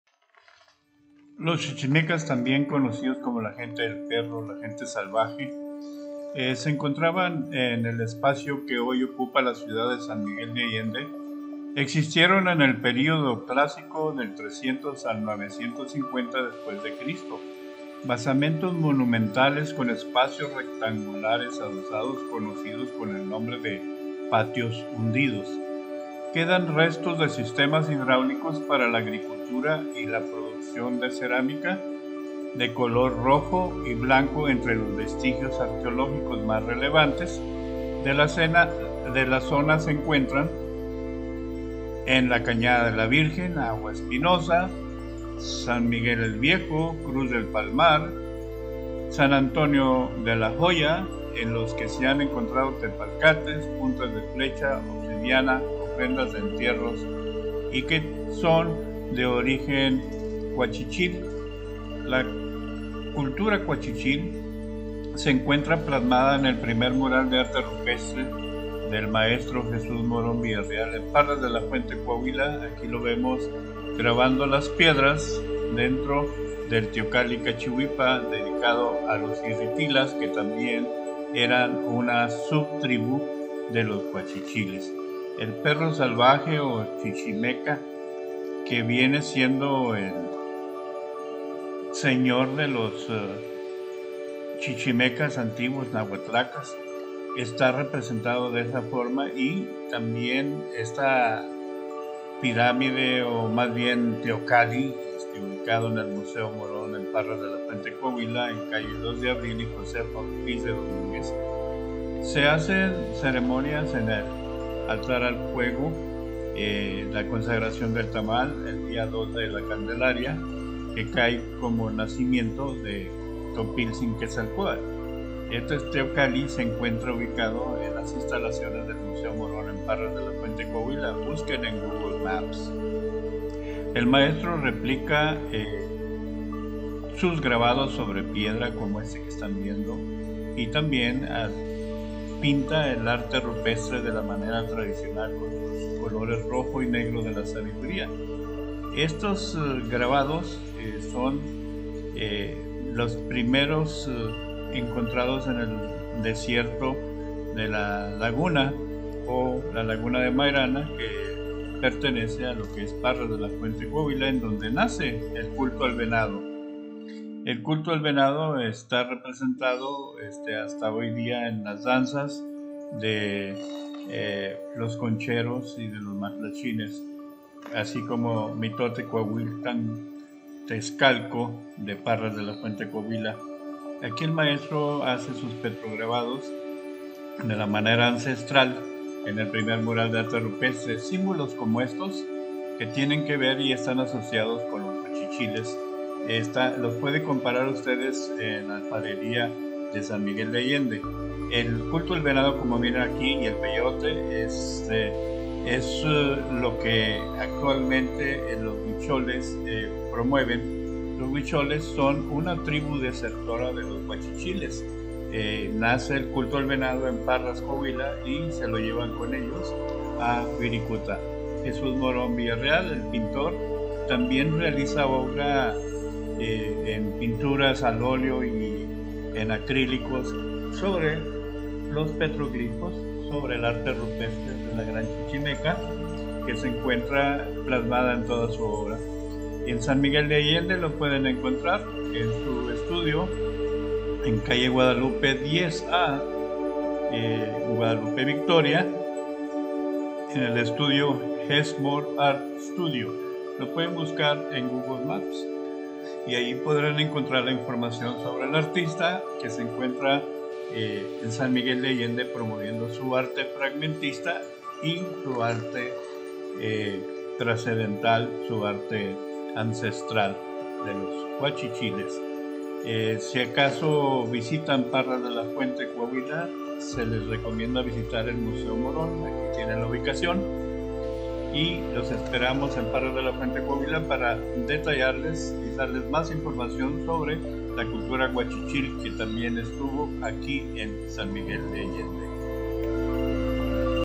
LOS GUACHICHILES DE SAN MIGUEL por Morón
The guachichila tribe settle down in old San Miguel de los chichimecas also in Parras de la Fuente Coahuila wher the cult to the deer were born and adopted by the wichol etnia, descendant of the Chichimeca- Iritila- Cuachichil by 950 DC. The Master painter and petrographer Jes Mor also known as Jesús Morón, uses the simbology of the ancestors chichimeca, as his way of expression.